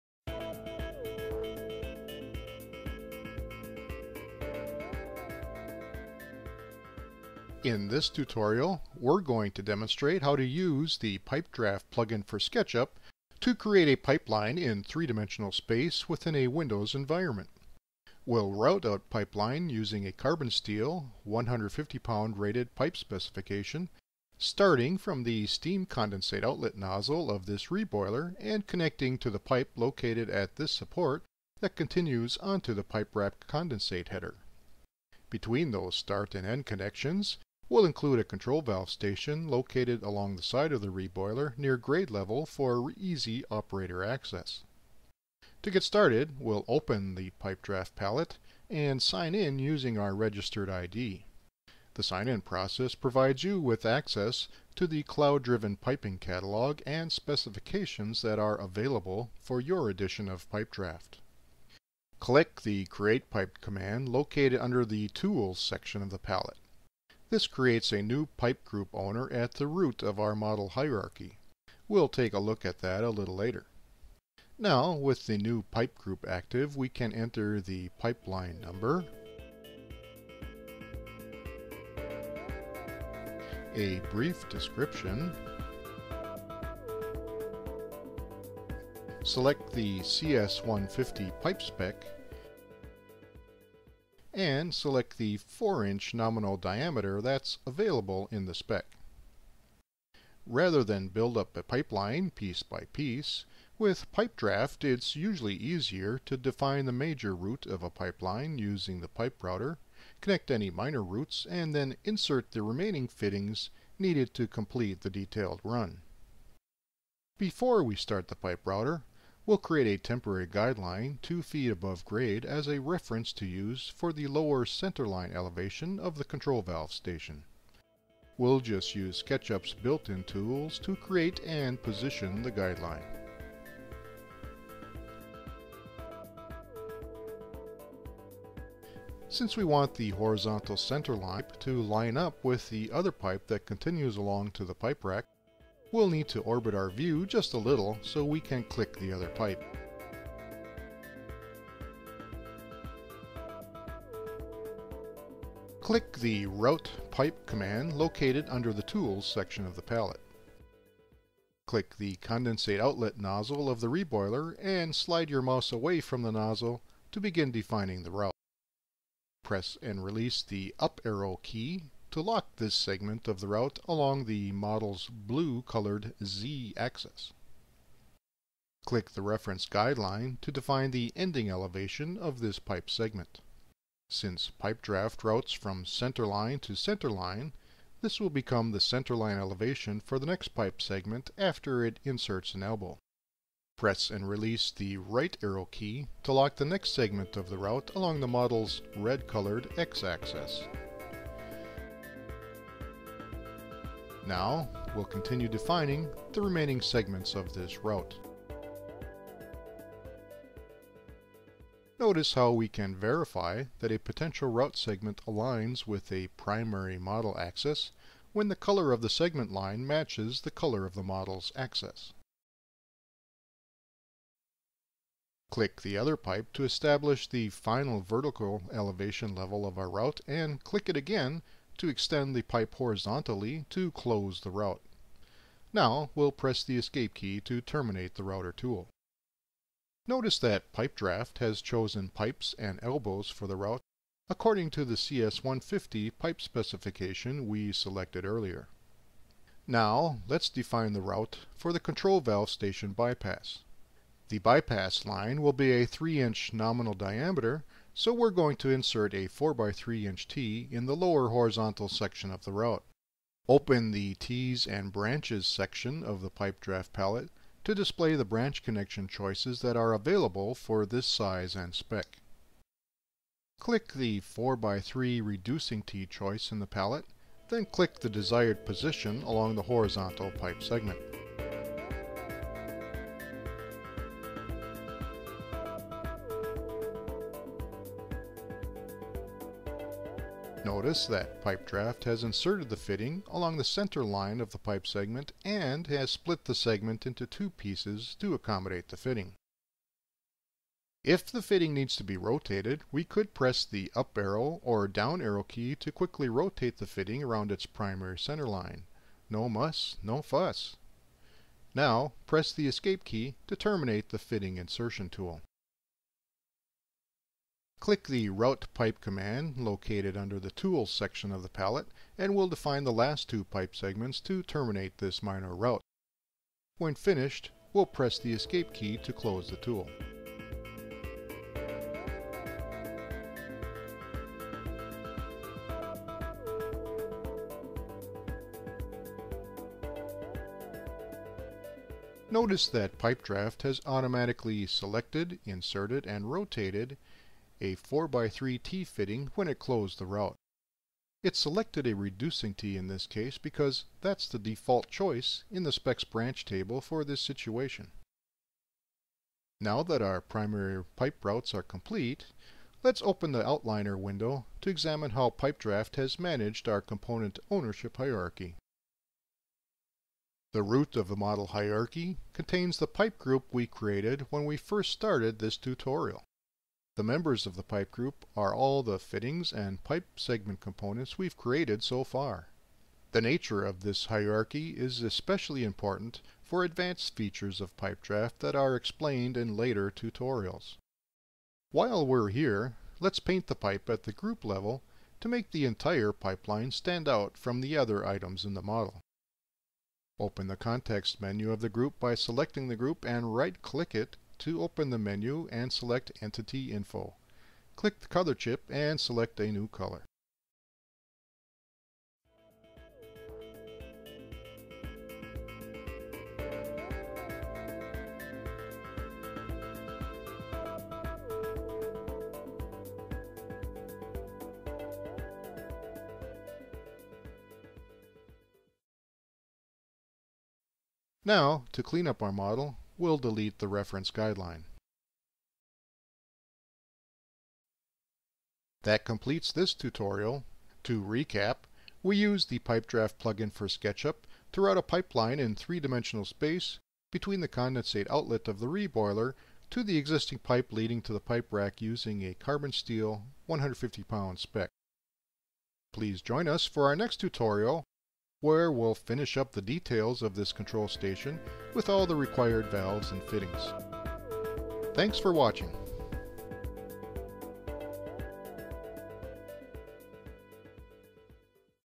Control Station Pipe Routing - Video Tutorial 2.1
Routing a Pipe Line for a Flow Control Valve Station in a Refinery using the PipeDraft Plug-in for SketchUp.